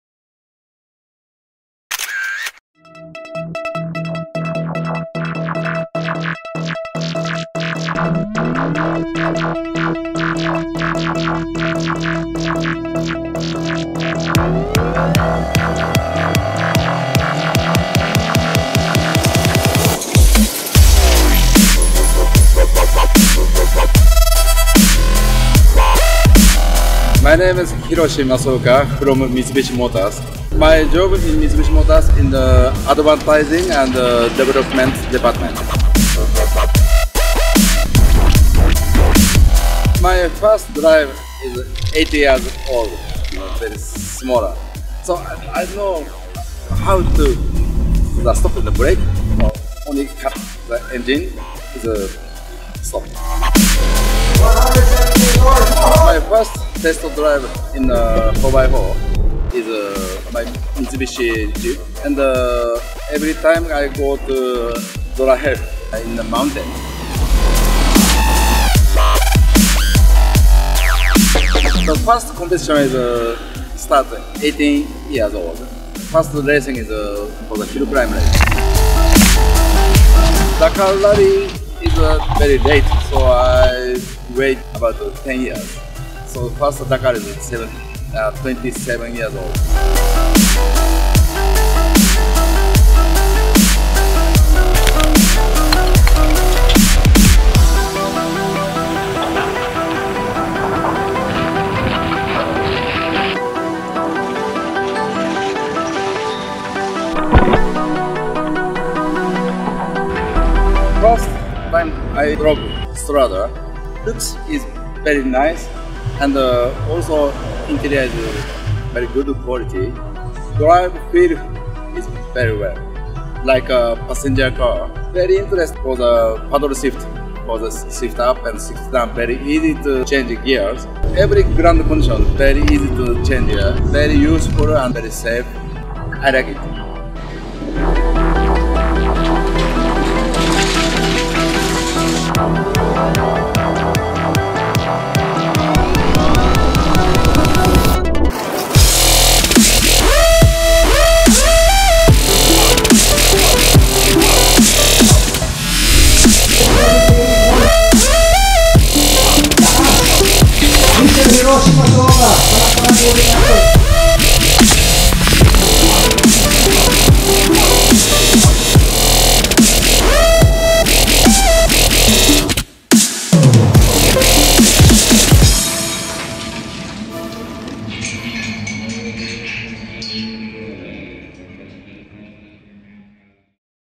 Hiroshi Masuoka Takes the All-New Mitsubishi Strada for a Spin
Dig cars?